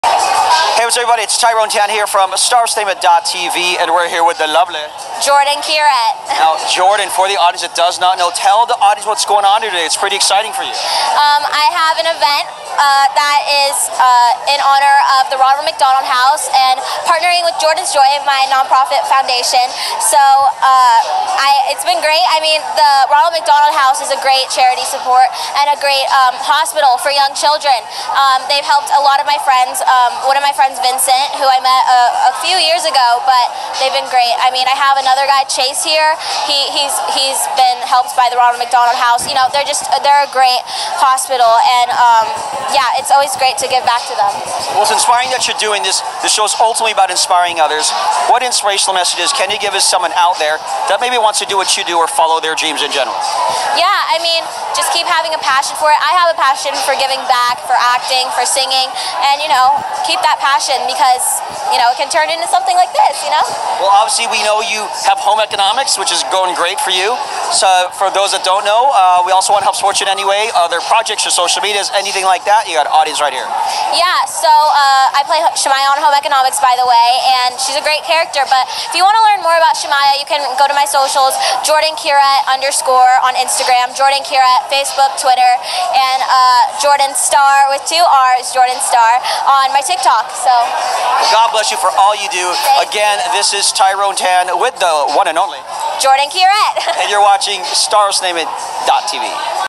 JORDYN CURET w/ TYRONE TANN - 2022 HALLOWEEN BASH, Benefitting the Ronald McDonald House L.A
JordynCuret HalloweenBash, Benefitting the RonaldMcDonaldHouse L.A.